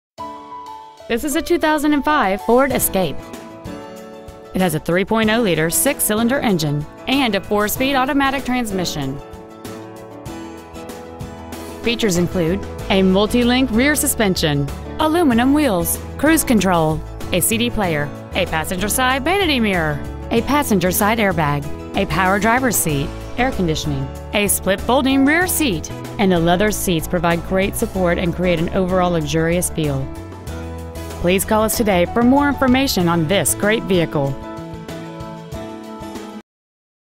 Pre-Owned 2005 FORD ESCAPE Plano TX
We have been honored to serve the Plano TX area , we promise that your experience at our dealership will exceed your expectations ! Year : 2005 Make : FORD Model : ESCAPE Engine : 3.0L V6 24V MPFI DOHC Trans . : 4-SPEED AUTOMATIC Exterior : REDFIRE CLEARCOAT METALLIC Miles : 100,389 Interior : MEDIUM / DARK FLINT Stock : WP2208A Five Star Ford of Plano W Plano Parkway Plano , TX 75093